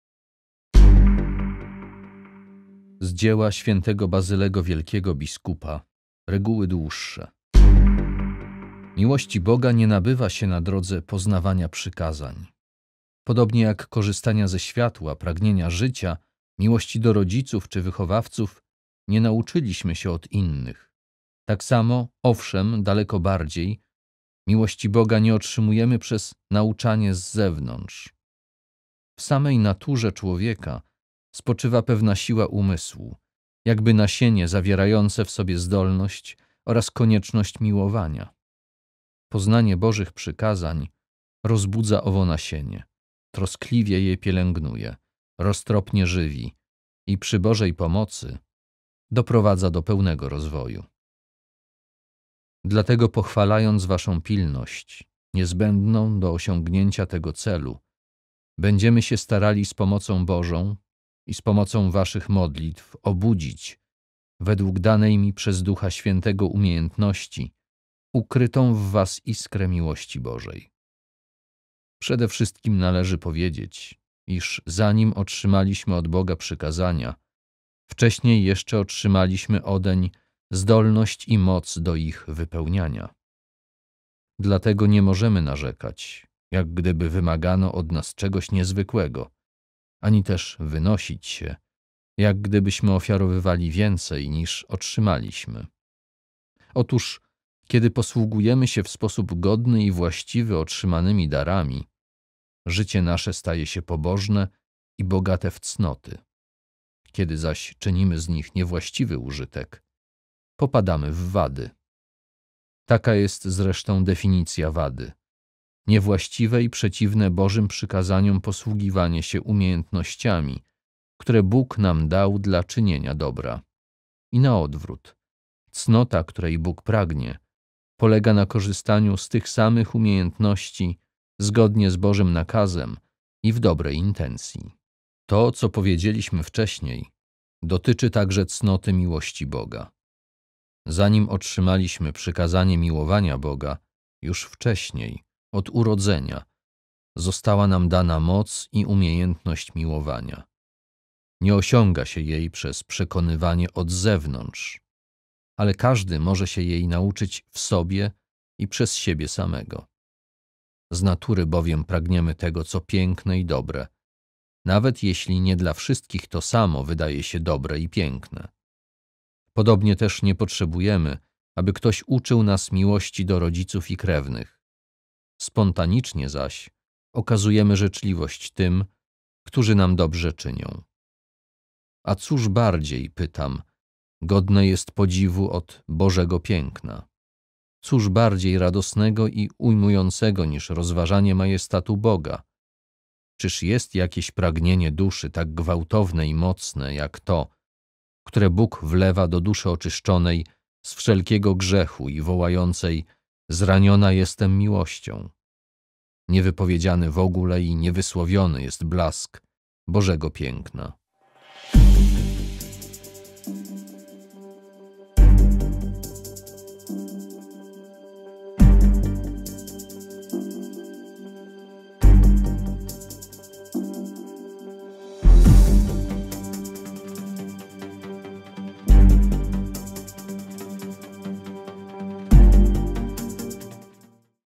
GodzinaCzytań | II Czytanie | 12 stycznia 2021
12 stycznia
I tydzień Okresu Zwykłego

GodzinaCzytań | II Czytanie
Z dzieła św. Bazylego Wielkiego, biskupa, Reguły dłuższe
Resp. 2, 1